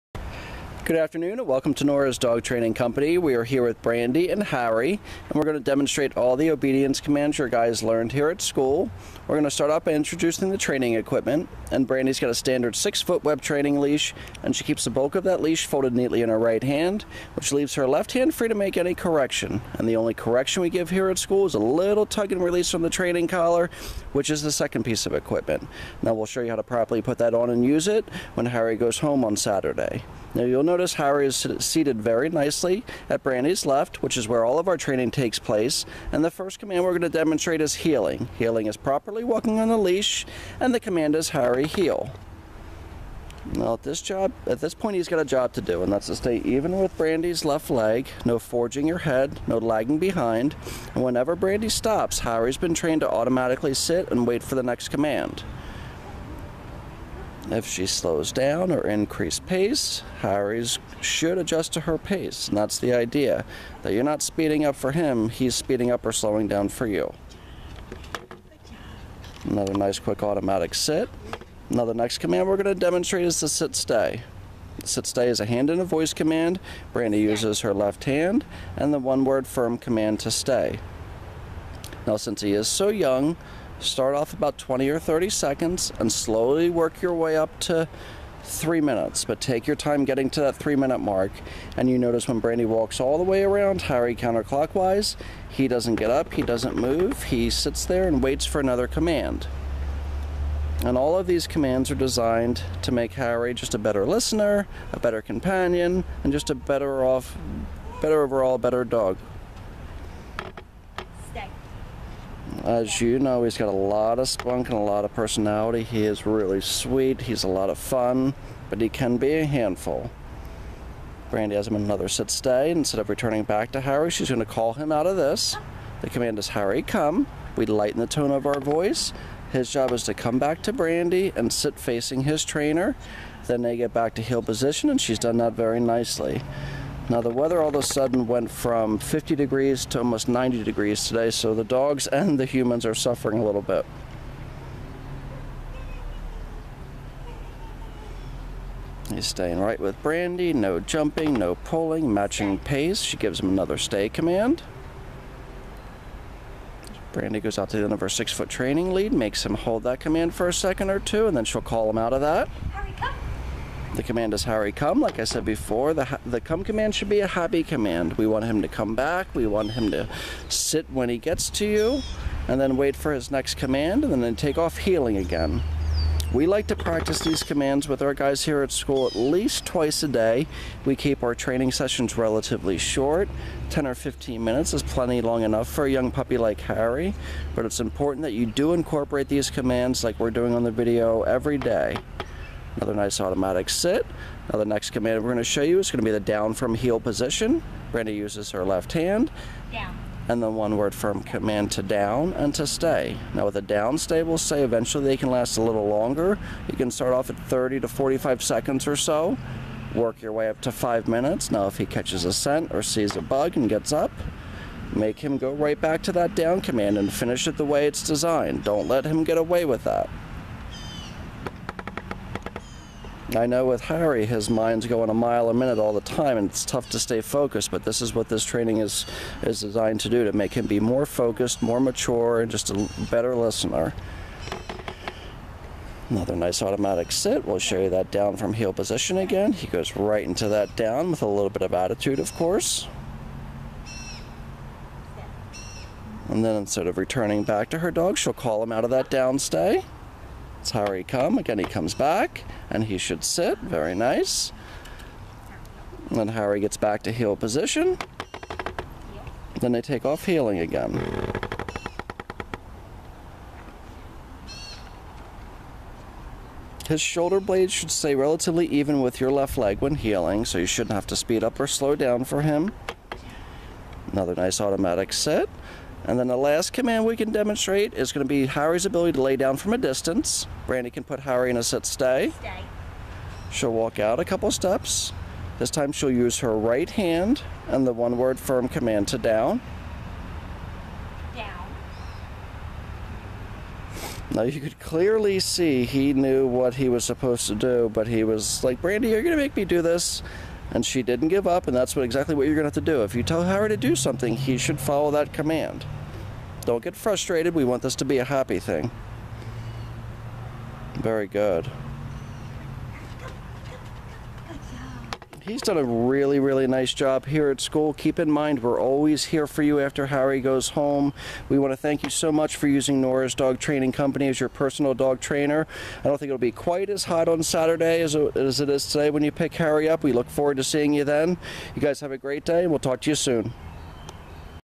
Harry
Harry is a sweet and funny little Carin Terrier puppy.  Harry has a lot of spunk and energy but loves showing off some of his new skills for his Mom.  Great job Harry !